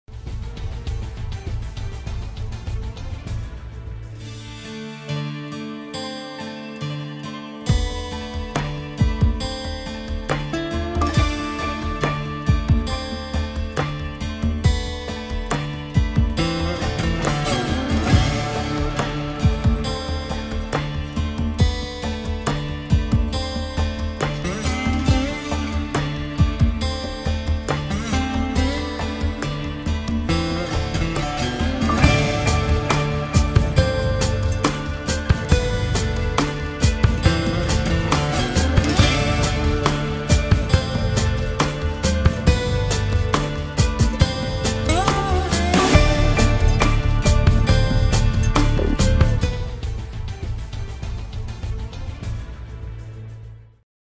مصر العربية | نجوم السياسة والفن في حفل انطلاق مبادرة الحبتور لدعم السينما
وسط حضور نجوم السياسة والفن، انطلقت مبادرة "الحبتورالثقافية لدعم صناعة السينما"، في حفل ضخم أقيم –مساء أمس الثلاثاء- في أحد الفنادق الكبرى بالقاهرة، تحت رعاية مجموعة الحبتور الإماراتية.